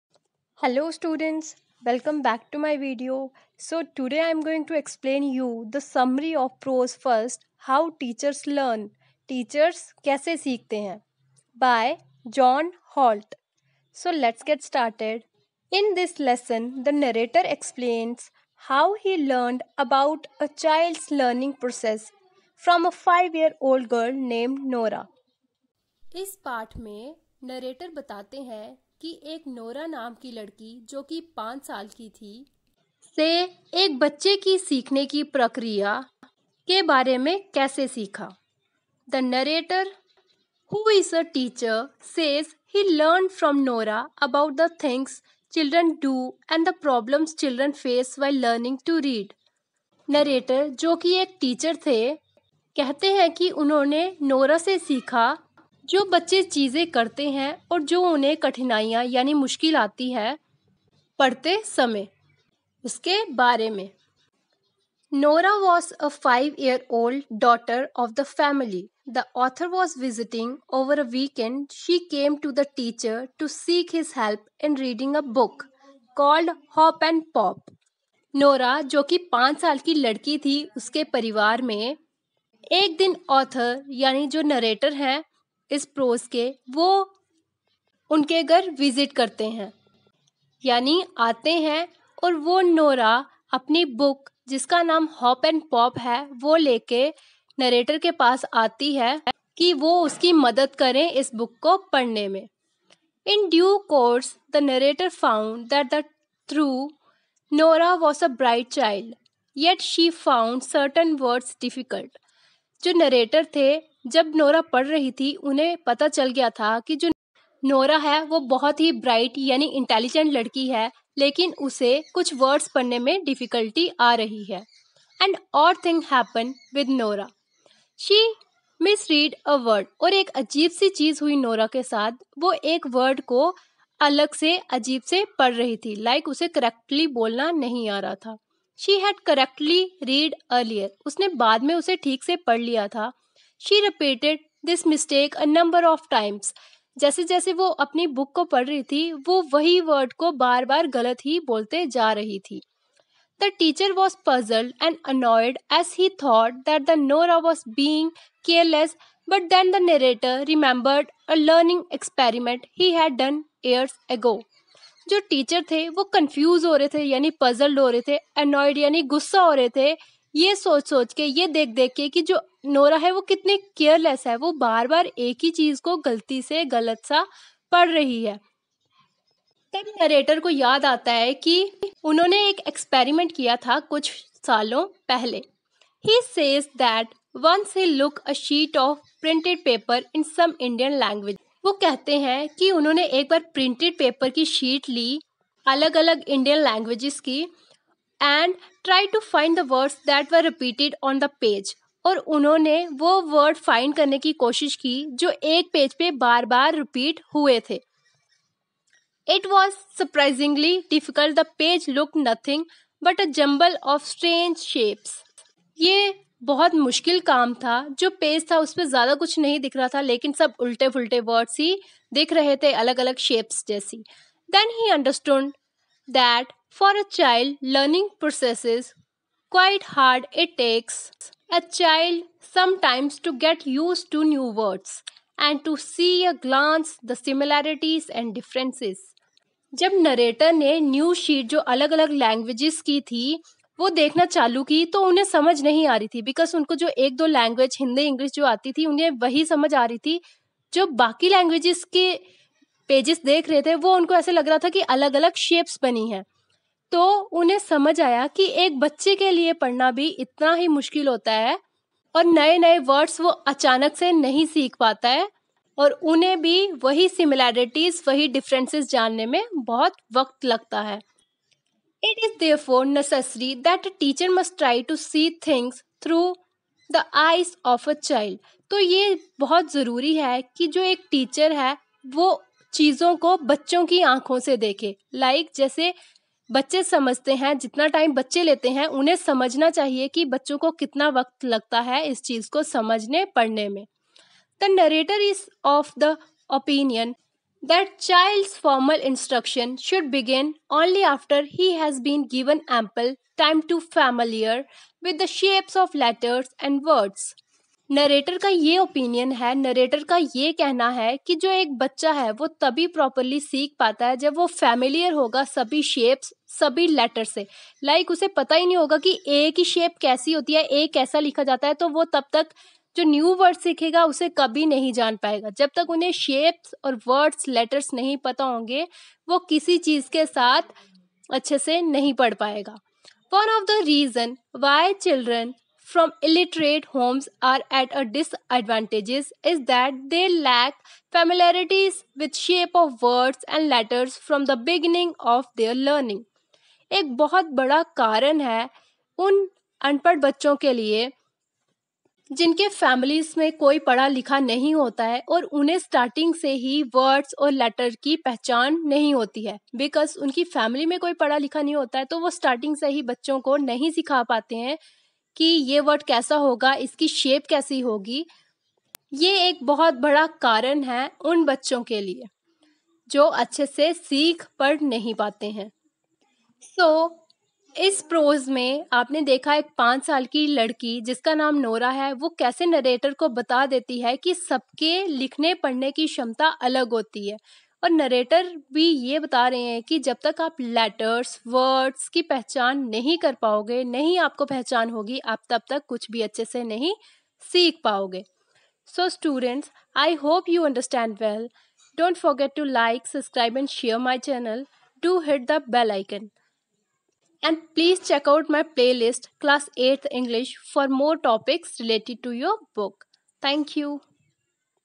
Summary of Prose-How Teachers Learn byJohnHolt|Hindi Explanation|Class8thEnglish|JKBose Tulip series
Checkout Playlist Class8thEnglish
For more Topics Related To Your Book